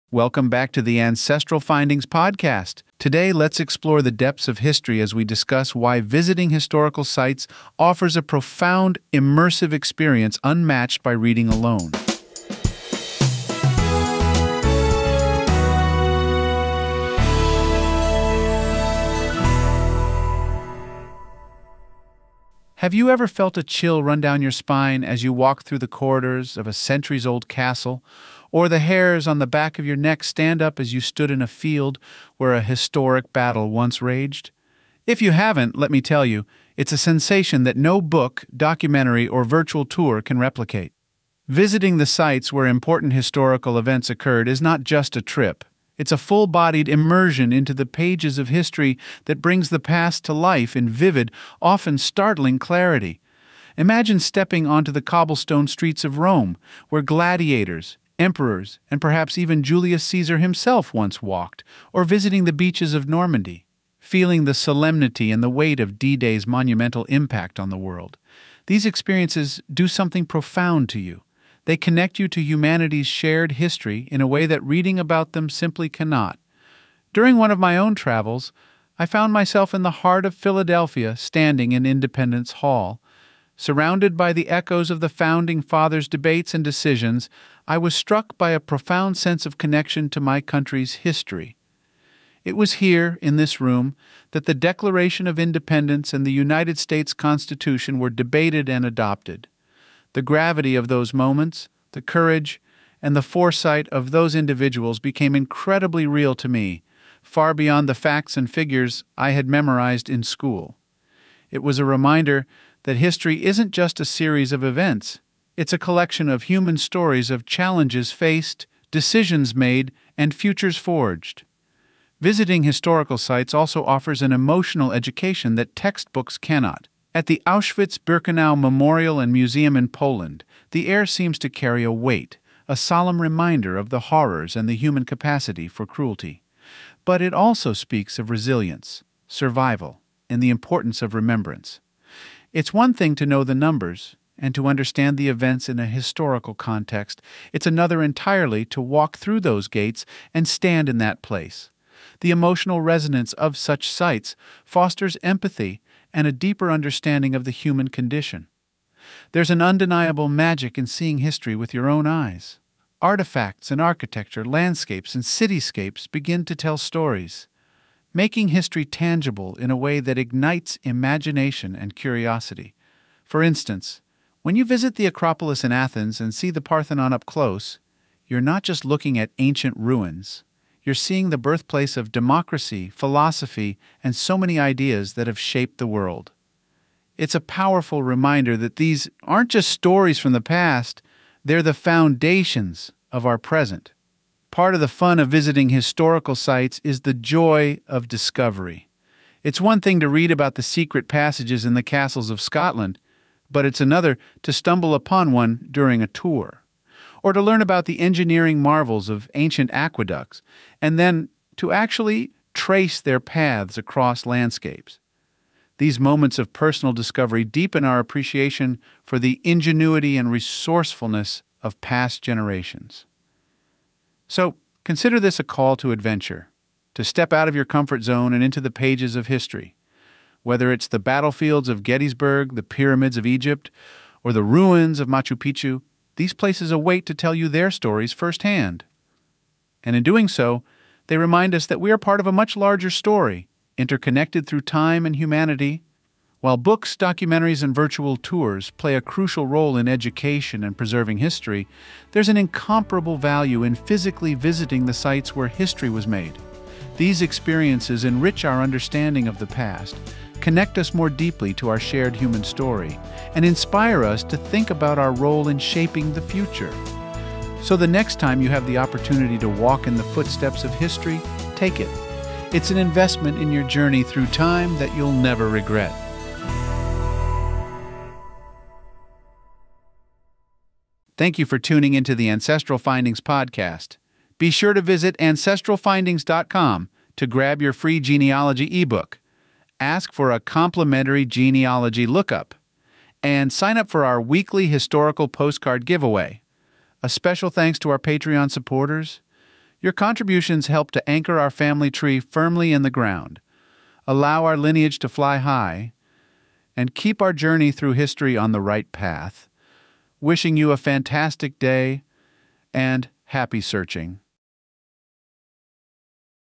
Why Stepping Into History Beats Just Reading About It | Ancestral Findings Podcast
AF-864: Experiencing History vs. Reading It  | Ancestral Findings Podcast
Experience history comes to life by stepping into its actual sites. Discover the captivating sensation of walking through centuries-old castles and battlefields.

Let’s explore history as we discuss why visiting historical sites offers an immersive experience unmatched by just reading alone.